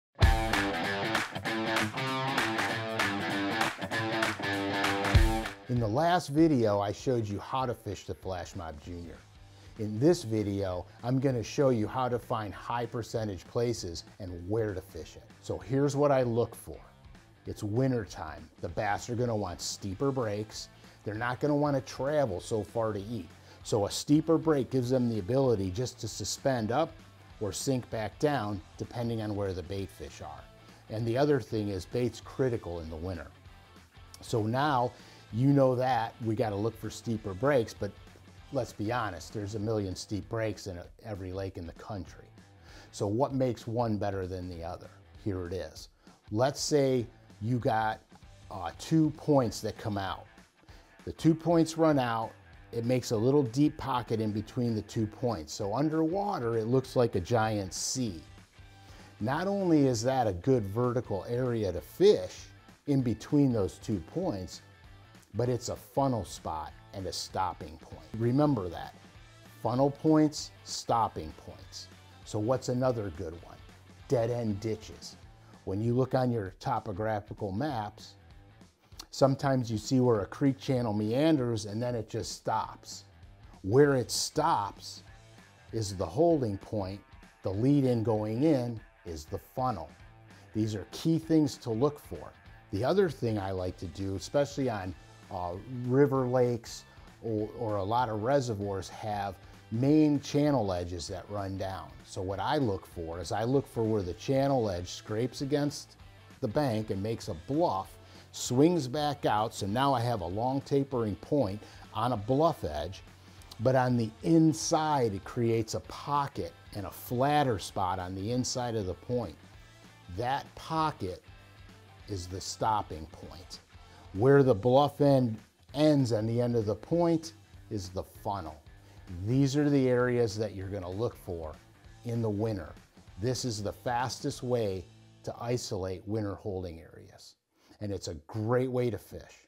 Winter Bass Location Secrets: Where to Fish The YUM Flash Mob Jr for Maximum Success - Bait School
Follow Frank Scalish as he showcases a master class how-to on where to fish the Flash Mob Jr. for maximum winter bass success!